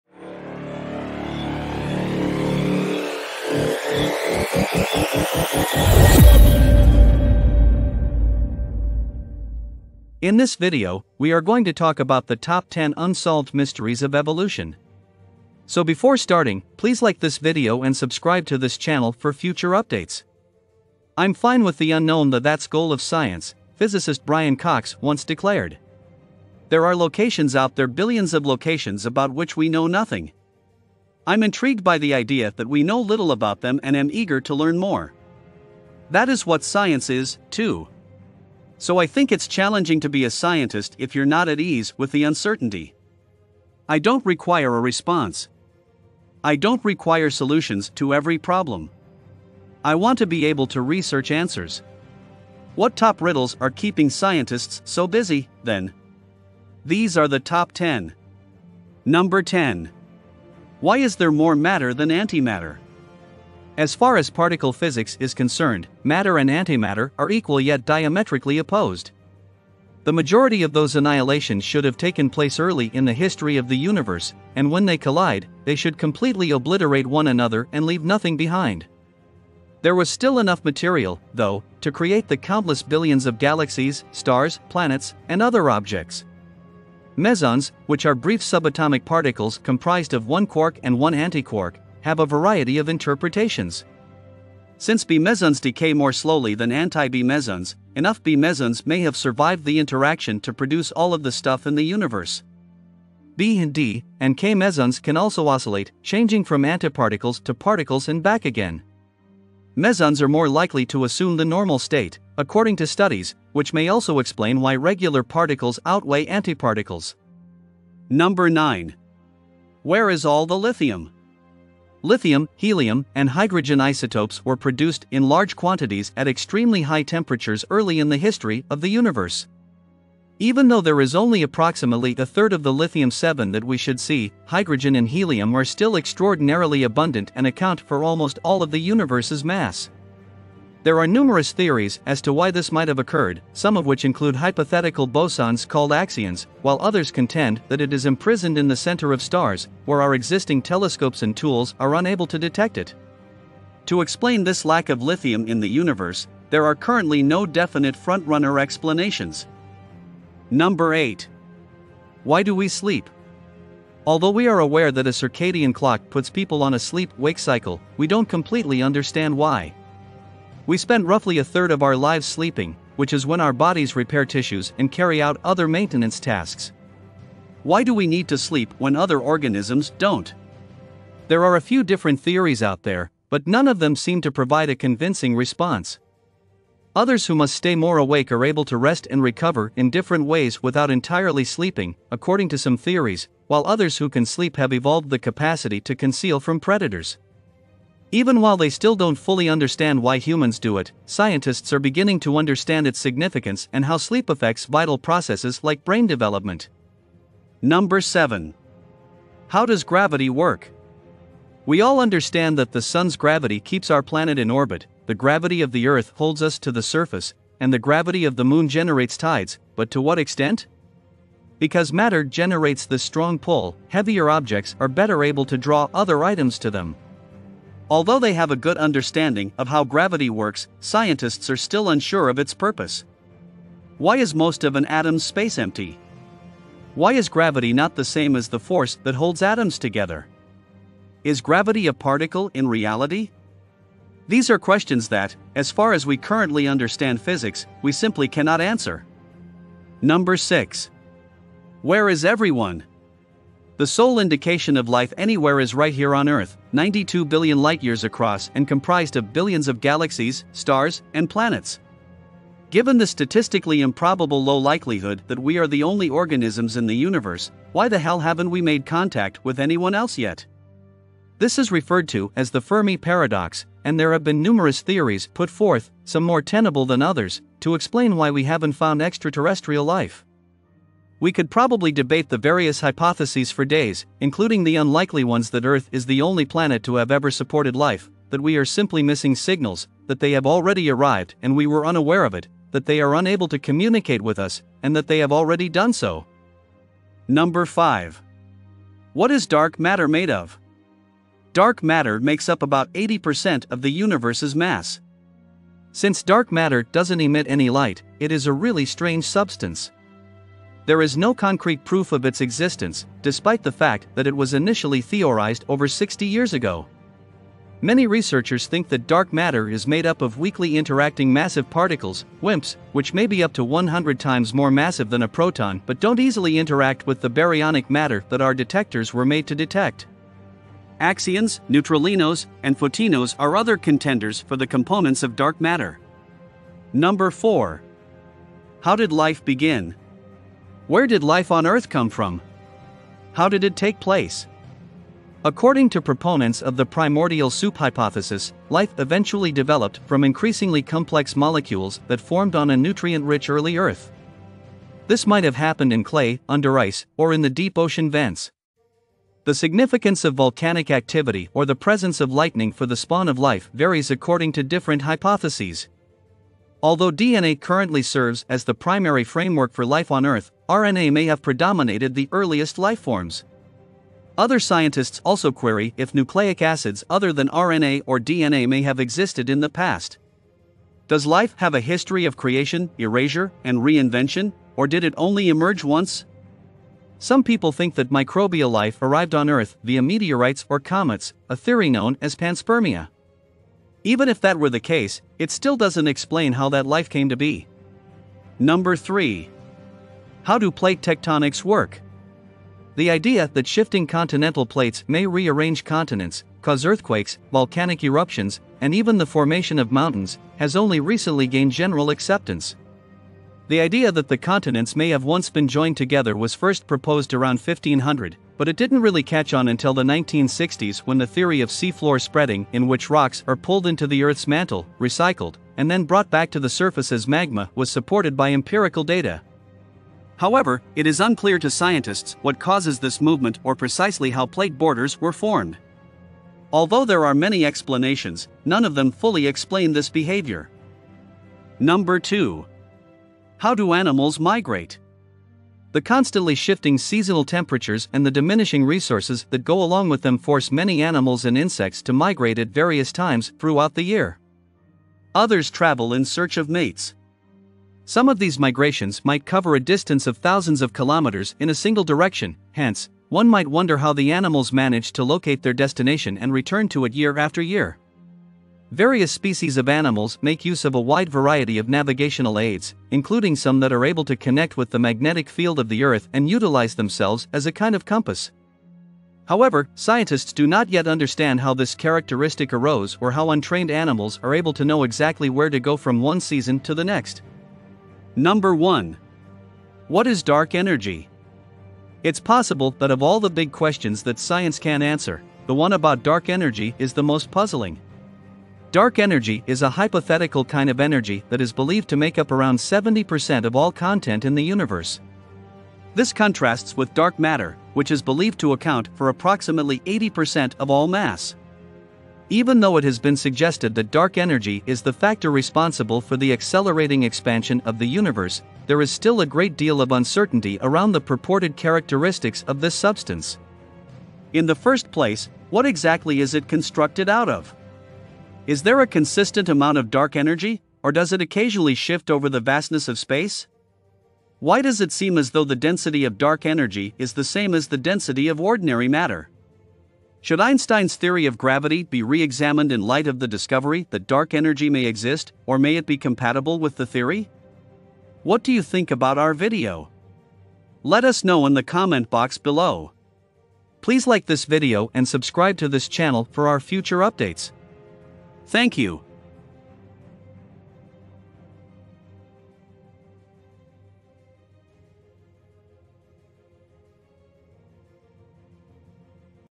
Top 10 Unsolved Mysteries of Evolution
The theory of evolution has been a source of fascination for centuries, but even with all that we know, there are still many mysteries that have yet to be solved. In this video, we'll be counting down the top 10 unsolved mysteries of evolution. From the origin of life on earth to the evolution of consciousness, these mysteries have confounded scientists and researchers for years. So join us as we delve into the unknown and explore some of the most perplexing questions surrounding the theory of evolution.